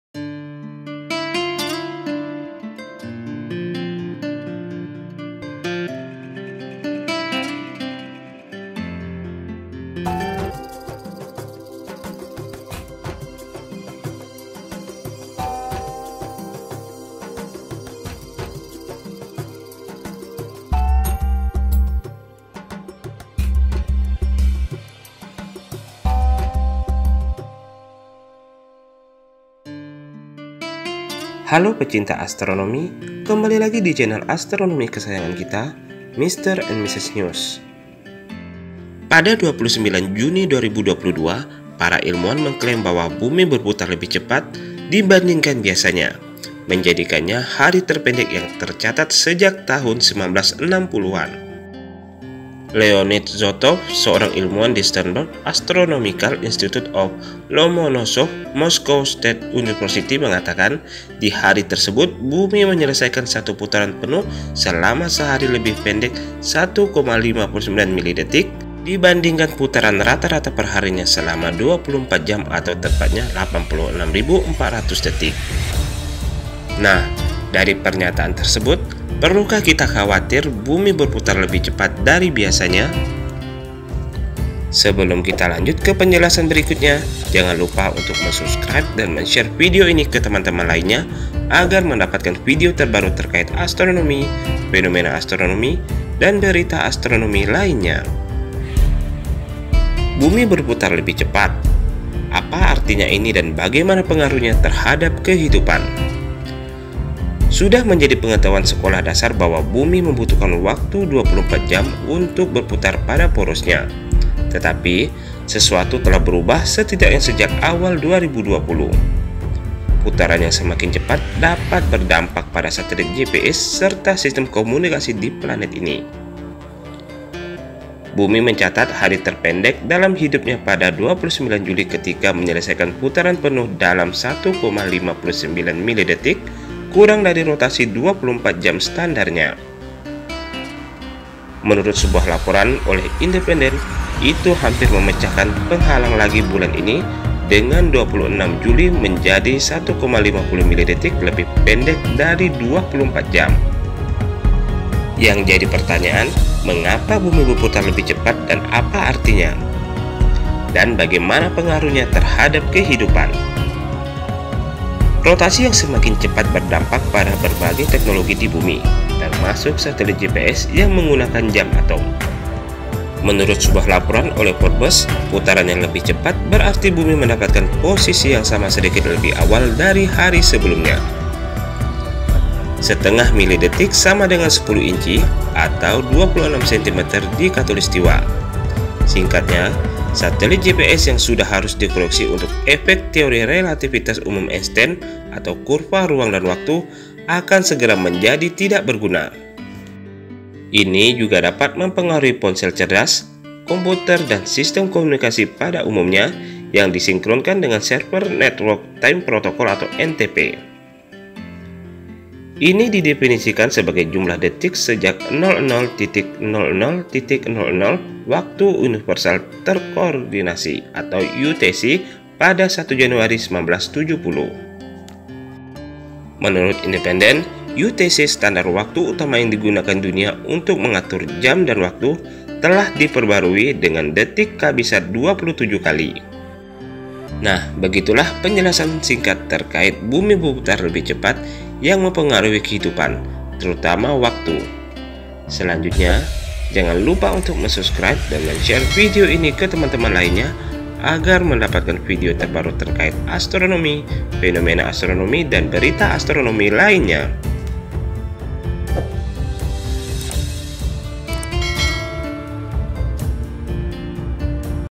BUMI BERPUTAR LEBIH CEPAT !! WAKTU BISA SALAH ?
TERIMAKASIH !!

Bumi mencatat hari terpendek dalam hidupnya pada 29 Juli ketika menyelesaikan putaran penuh dalam 1,59 milidetik kurang dari rotasi 24 jam standarnya.Menurut sebuah laporan oleh Independent, itu hampir memecahkan penghalang lagi bulan ini, dengan 26 Juli menjadi 1,50 milidetik lebih pendek dari 24 jam.Mengapa Bumi berputar lebih cepat dan apa artinya, mari kita cari tahu:Bagaimana pengaruhnya terhadap kehidupan?Rotasi yang semakin cepat berdampak pada berbagai teknologi di Bumi, termasuk satelit GPS yang menggunakan jam atom.Menurut sebuah laporan oleh Forbes, putaran yang lebih cepat berarti Bumi mendapatkan posisi yang sama sedikit lebih awal dari hari sebelumnya. Setengah milidetik sama dengan 10 inci atau 26 sentimeter di khatulistiwa. Singkatnya, satelit GPS—yang sudah harus dikoreksi untuk efek teori relativitas umum Einstein (kurva ruang dan waktu)—akan segera menjadi tidak berguna.Ini juga dapat memengaruhi ponsel cerdas, komputer, dan sistem komunikasi pada umumnya, yang disinkronkan dengan server Network Time Protocol (NTP). Ini didefinisikan sebagai jumlah detik sejak 00:00:00 Waktu Universal Terkoordinasi (UTC) pada 1 Januari 1970.